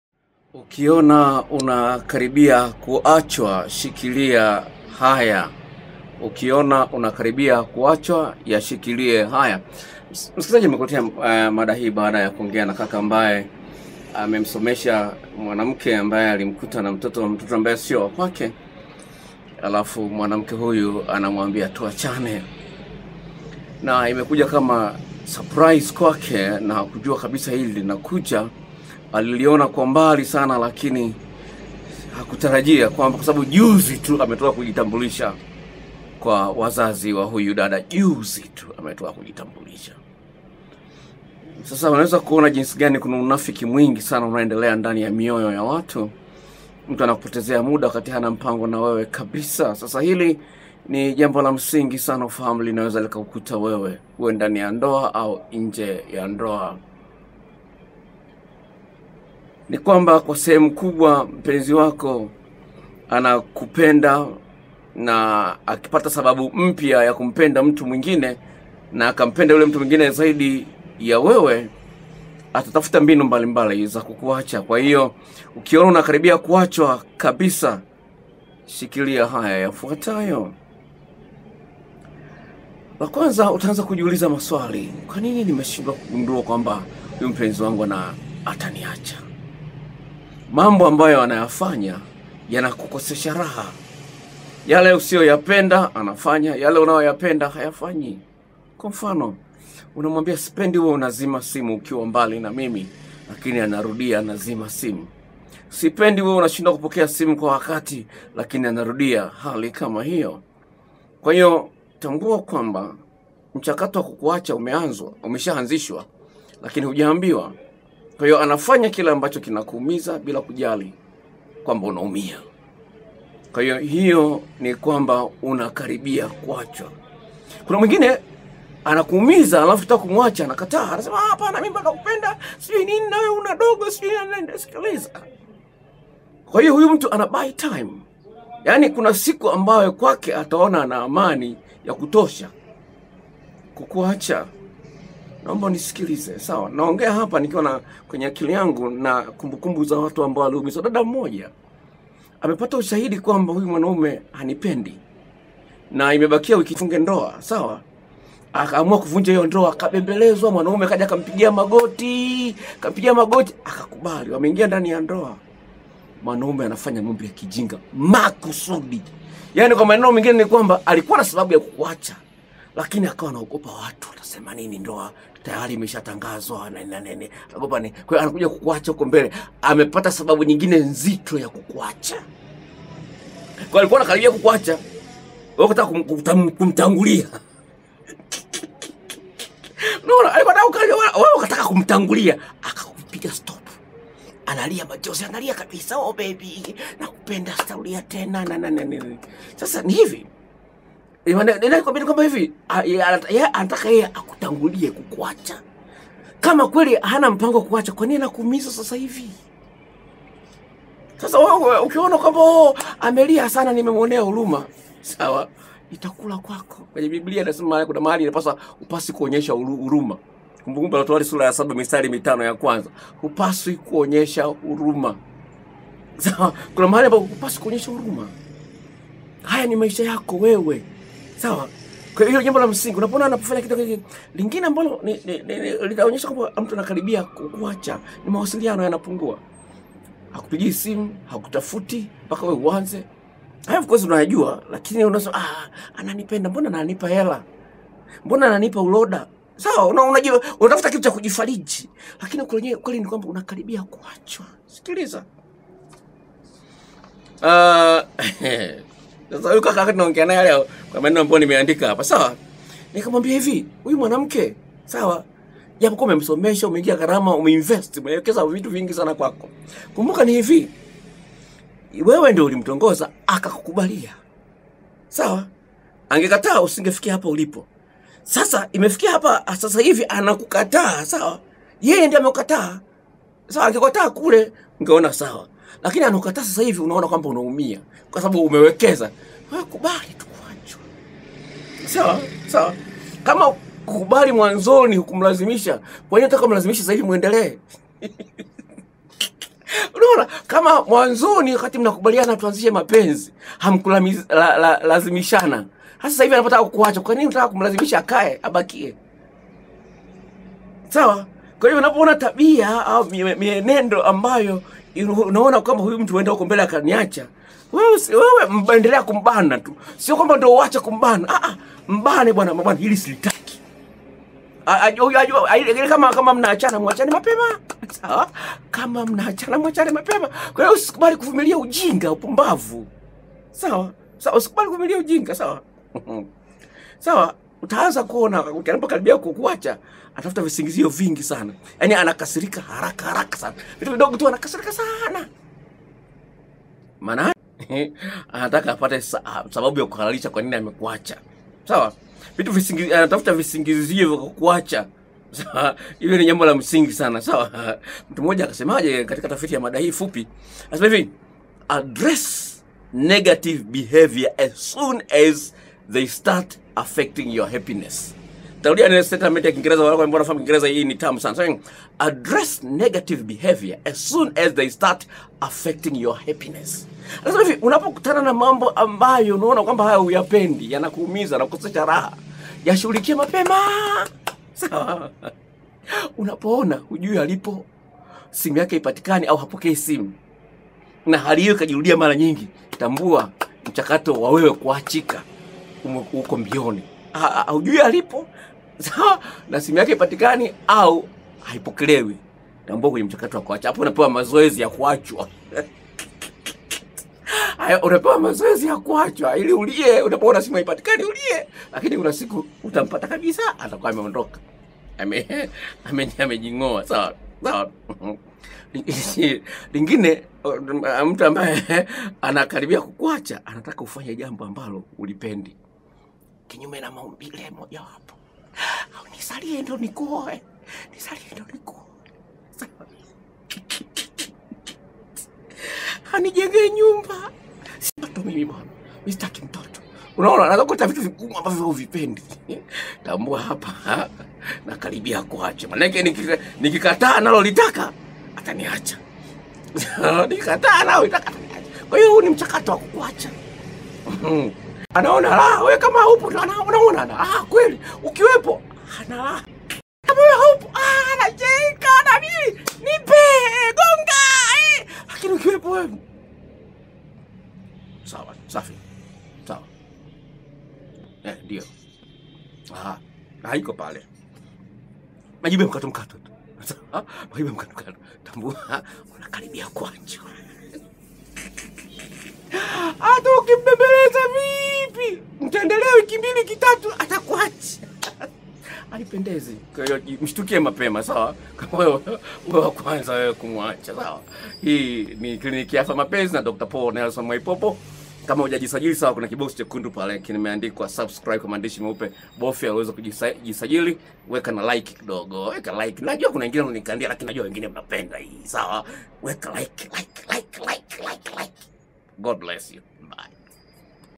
Ukiona dalili za kukaribia kuachwa na mpenzi shikilia haya
Dalil ya mvua ni mawingu na penye moshi pana moto,kuwa makini usijejuta kwa kupotezewa muda TUWASILIANE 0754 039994